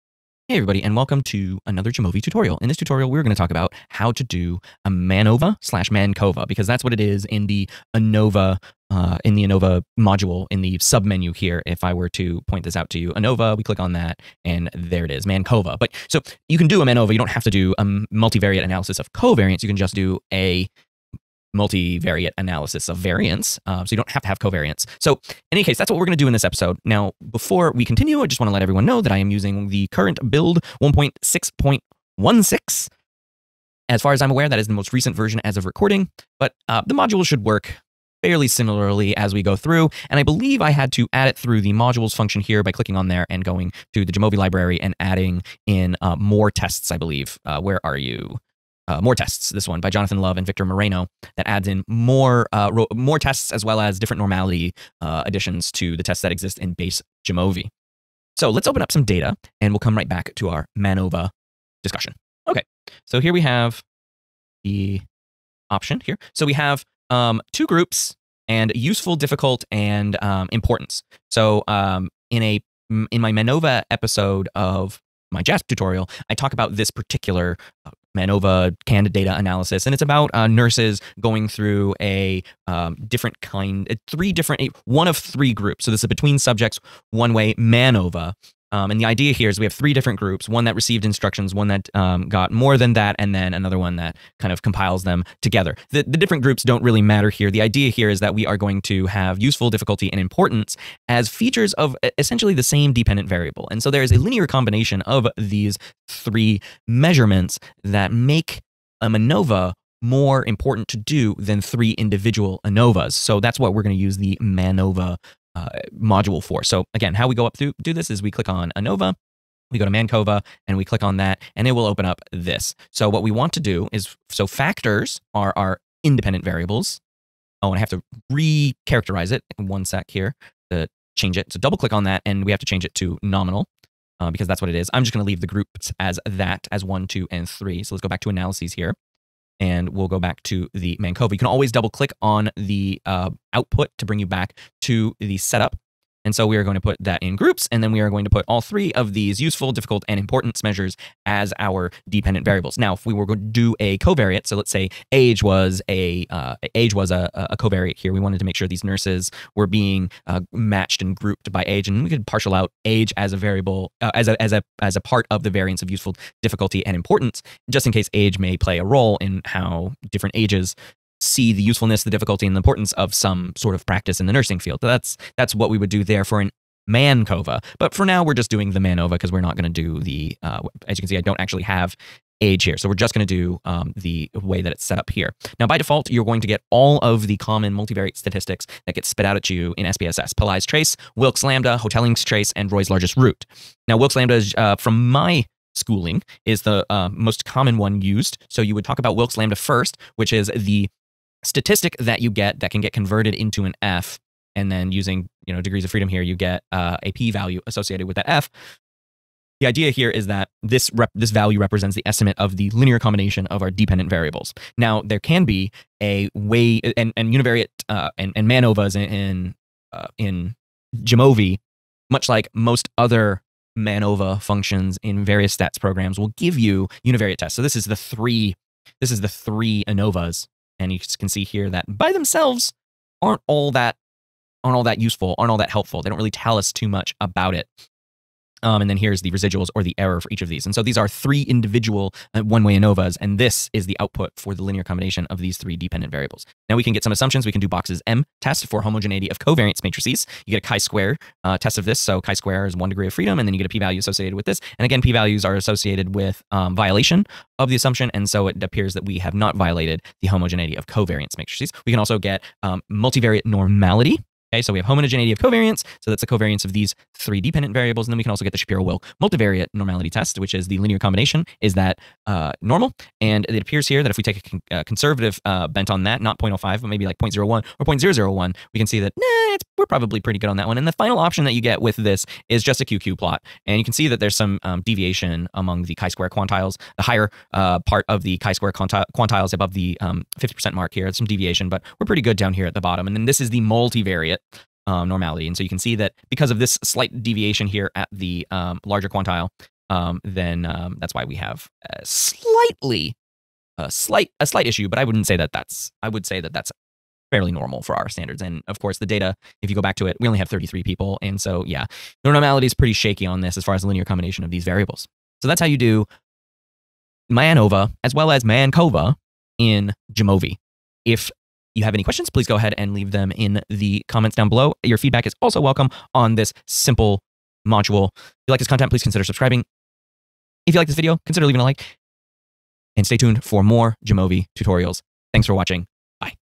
Jamovi 1.2/1.6 Tutorial: MANOVA/MANCOVA (Episode 20)
In this Jamovi tutorial, I go through a quick example on how to use the MANOVA/MANCOVA module/function that is found by adding the "More tests" module from the package library. Options include choosing which multivariate test to include and which assumption checks to include.

NOTE: This tutorial uses the current build of Jamovi, 1.6.16 on MacOS. Version 1.6 contains all the new features to the program as of this recording date. Version 1.6 is listed as their most stable build.